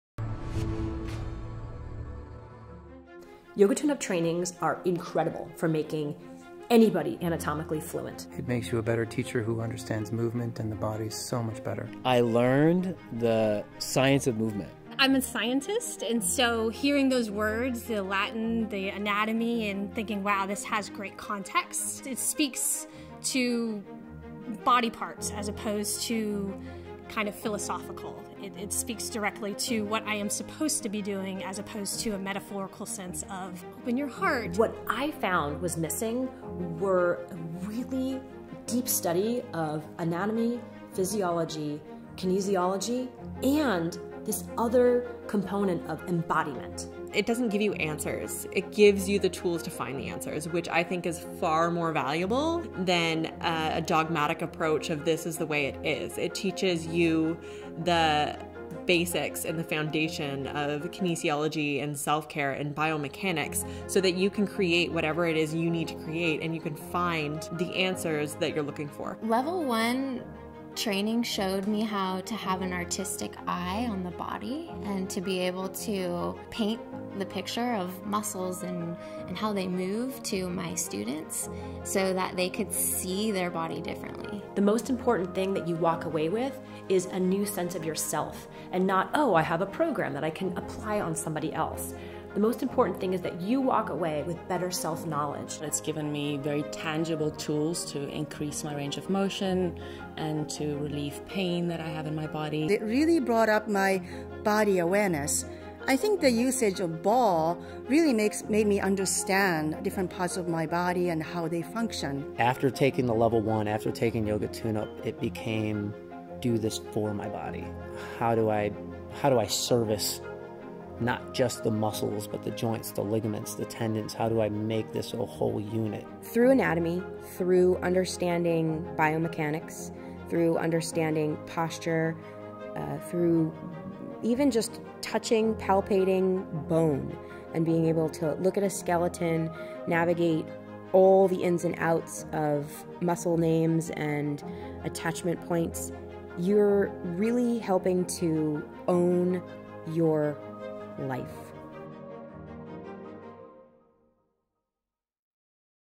Yoga Tune Up® Teacher Training Certification – A Science Based Approach
The Yoga Tune Up® Certification Training takes a science-based approach to corrective exercise, striking a balance between the worlds of yoga, fitness, athletics, pain management, and myofascial self-care. Gain anatomical fluency as you learn the science of movement to help increase range of motion, relieve pain and improve overall body awareness.